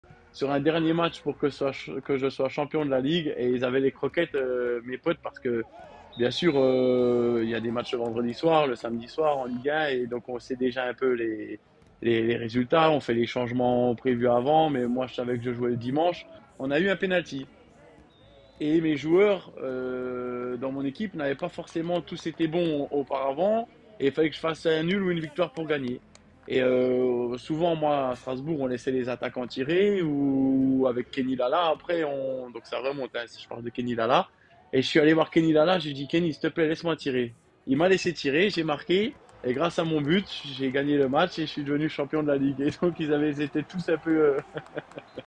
L'anecdote de Lienard sur MPG ⚽️
Il a réussi à tirer un péno pour gagner sa ligue